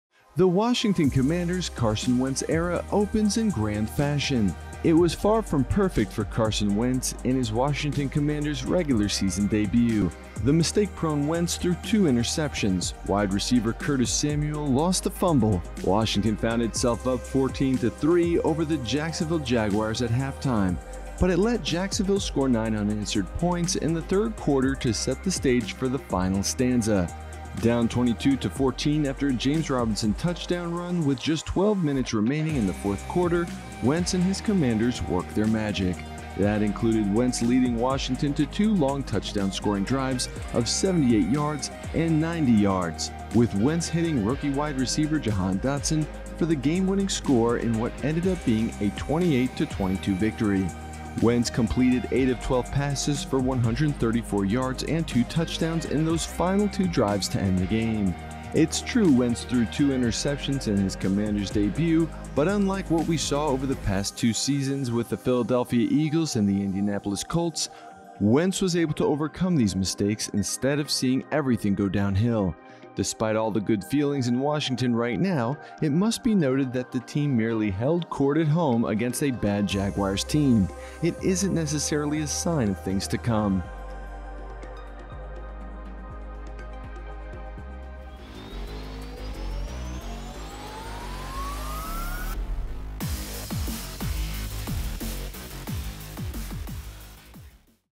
Carson Wentz era with Washington Commanders opens up in grand fashion
Down 22-14 after a James Robinson touchdown run with just under 12 minutes remaining in the fourth quarter, Wentz and his Commanders worked their magic. That included Wentz leading Washington two long touchdown-scoring drives of 78 yards and 90 yards, respectively. The dude was on absolute fire to close things out for Washington. That included Wentz hitting rookie wide receiver Jahan Dotson for the game-winning score in what ended up being a 28-22 victory.